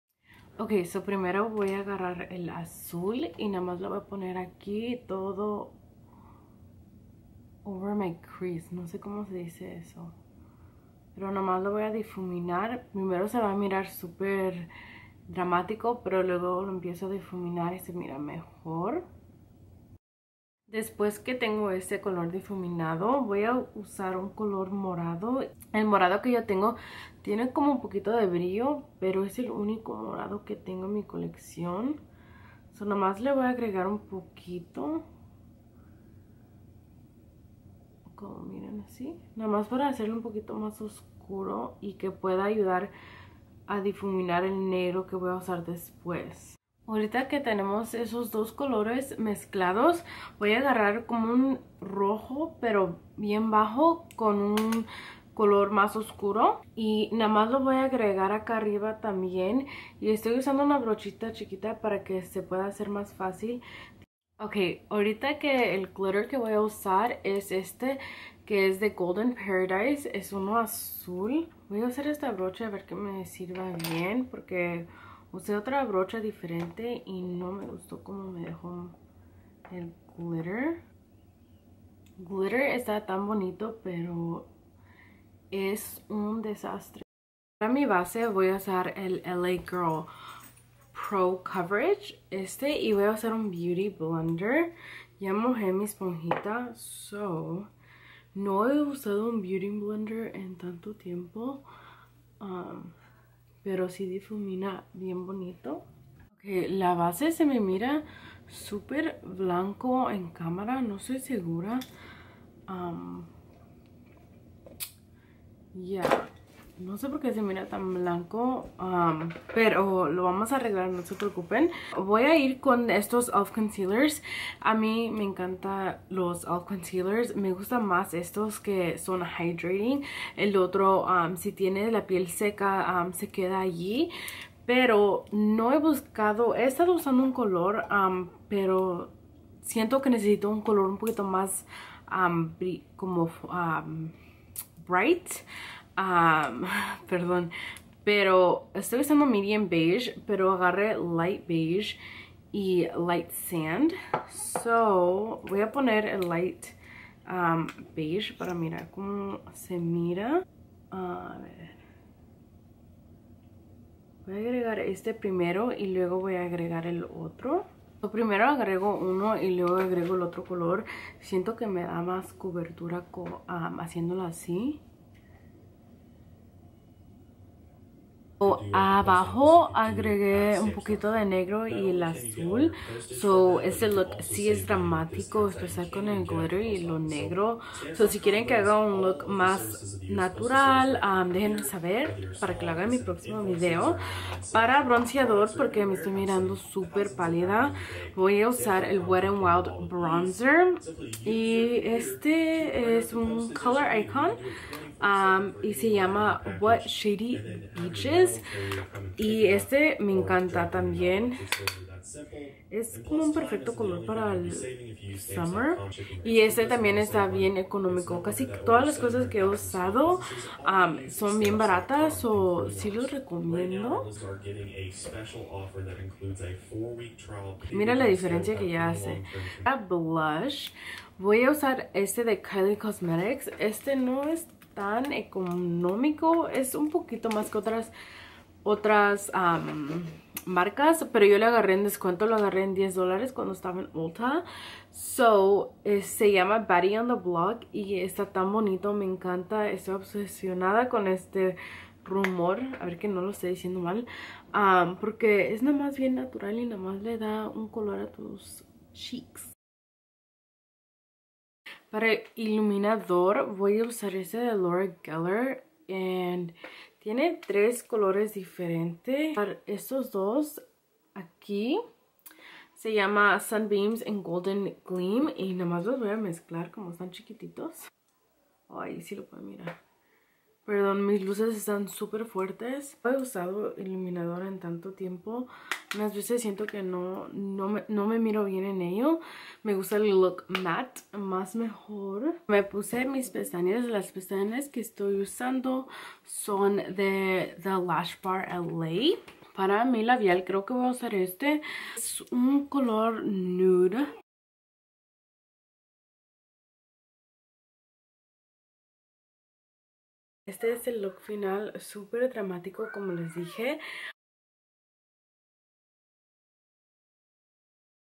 Tutorial De Maquillaje Azul escarchado
Gracias por mirar mi video! No se les olvide subscribirse 💙
Dejen me saber otros video que les gustaría mirar.

Eyeshadow / Palette : @Morphebrushes x @Jaclynhill palette
Glitter: @goldenparadise__ Goldernparadise__ in Blue moon
Lashes / Pestañas : @thelashbarla
Foundation / Base: @lagirlcosmetics pro coverage HD
Concealer / Corrector : @elfcosmetics in hydrating camo concealer
Bronzer / Bronziador : @wetnwildbeauty in what shady beaches
Highlighter / Iluminador : @lauragellerbeauty in sun beam & golden beam
Lips / Labios : @milanicosmetics